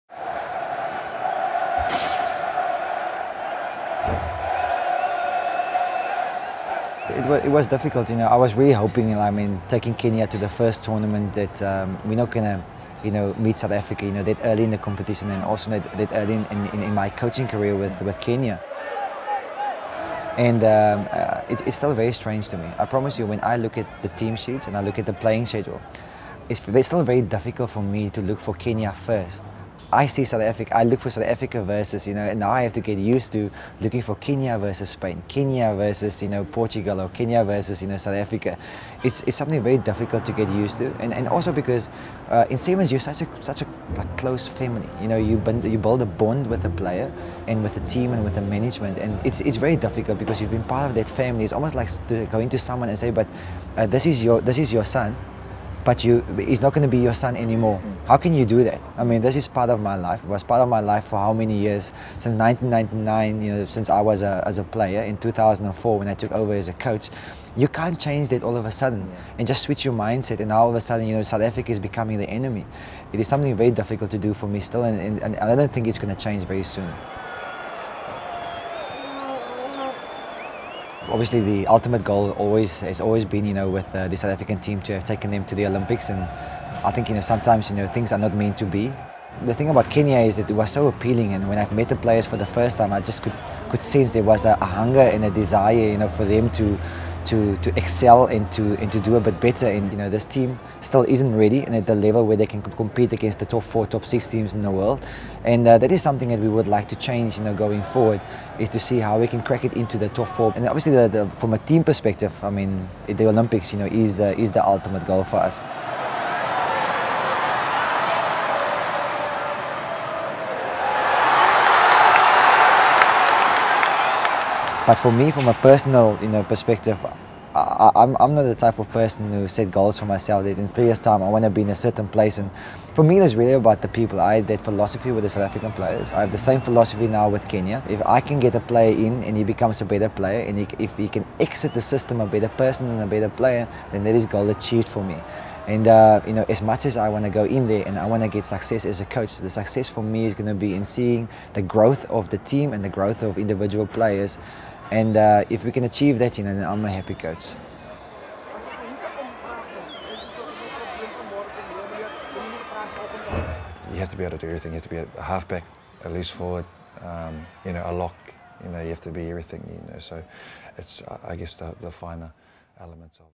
SA Rugby Spotlight: Paul Treu
Former Blitzbokke coach Paul Treu - now head of the Kenyan Sevens side - talks exclusively to SA Rugby Magazine about being on the other side of the fence, and his goals for Kenya.